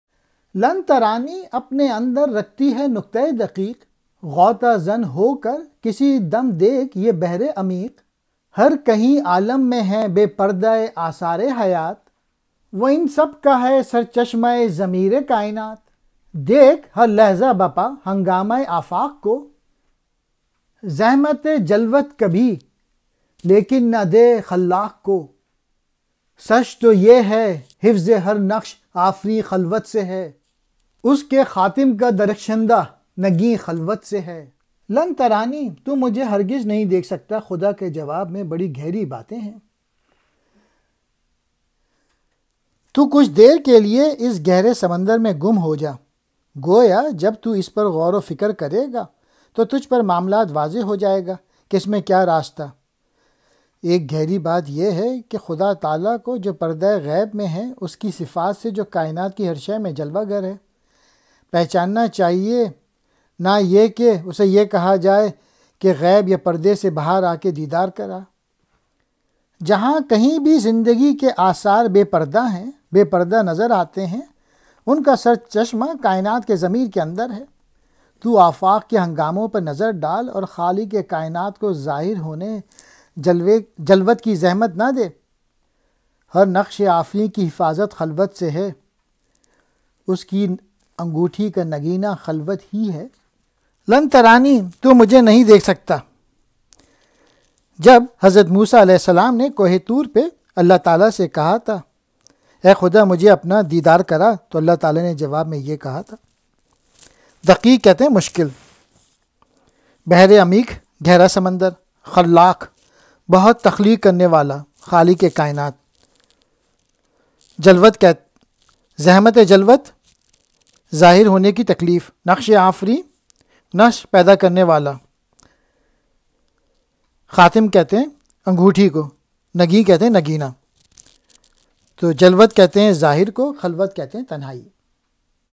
Mohkamat-e-Alam-e-Qurani | Khilafat-e-Adam,Javed Nama21-9 | خلافت آدم | جاوید نامہ
خلافت آدم
در دو عالم ھر کجا آثار عشق
ابن آدم سری از اسرار عشق
سر عشق از عالم ارحام نیست
او ز سام و حام و روم و شام نیست
کوکب بی شرق و غرب و بی غروب
در مدارش نی شمال و نی جنوب
حرف انی جاعل تقدیر او
از زمین تا آسمان تفسیر او
مرگ و قبر و حشر و نشر احوال اوست
نور و نار آن جہان اعمال اوست
او امام و او صلوت و او حرم
او مداد و او کتاب و او قلم
خردہ خردہ غیب او گردد حضور
نی حدود او را نہ ملکش را ثغور
از وجودش اعتبار ممکنات
اعتدال او عیار ممکنات
من چہ گویم از یم بی ساحلش
غرق اعصار و دہور اندر دلش
آنچہ در آدم بگنجد عالم است
آنچہ در عالم نگنجد آدم است
آشکارا مہر و مہ از جلوتش
نیست رہ جبریل را در خلوتش
برتر از گردون مقام آدم است
اصل تہذیب احترام آدم است

زندگی ای زندہ دل دانی کہ چیست
عشق یک بین در تماشای دوئی است
مرد و زن وابستۂ یکدیگرند
کائنات شوق را صورتگرند
زن نگہدارندۂ نار حیات
فطرت او لوح اسرار حیات
آتش ما را بجان خود زند
جوہر او خاک را آدم کند
در ضمیرش ممکنات زندگی
از تب و تابش ثبات زندگی
شعلہ ئی کز وی شرر ہا در گسست
جان و تن بی سوز او صورت نبست
ارج ما از ارجمندیھای او
ما ہمہ از نقشبندیھای او
حق ترا داد است اگر تاب نظر
پاک شو قدسیت او را نگر

ای ز دینت عصر حاضر بردہ تاب
فاش گویم با تو اسرار حجاب
ذوق تخلیق آتشی اندر بدن
از فروغ او فروغ انجمن
ہر کہ بردارد ازین آتش نصیب
سوز و ساز خویش را گردد رقیب
ہر زمان بر نقش خود بندد نظر
تا نگیرد لوح او نقش دگر
مصطفی اندر حرا خلوت گزید
مدتی جز خویشتن کس را ندید
نقش ما را در دل او ریختند
ملتی از خلوتش انگیختند
می توانی منکر یزدان شدن
منکر از شأن نبی نتوان شدن
گرچہ داری جان روشن چون کلیم
ہست افکار تو بی خلوت عقیم
از کم آمیزی تخیل زندہ تر
زندہ تر جویندہ تر یابندہ تر

علم و ہم شوق از مقامات حیات
ہر دو می گیرد نصیب از واردات
علم از تحقیق لذت می برد
عشق از تخلیق لذت می برد
صاحب تحقیق را جلوت عزیز
صاحب تخلیق را خلوت عزیز
چشم موسی خواست دیدار وجود
این ہمہ از لذت تحقیق بود
لن ترانے نکتہ ہا دارد رقیق
اندکے گم شو درین بحر عمیق
ہر کجا بی پردہ آثار حیات
چشمہ زارش در ضمیر کائنات
در نگر ہنگامۂ آفاق را
زحمت جلوت مدہ خلاق را
حفظ ہر نقش آفرین از خلوت است
خاتم او را نگین از خلوت است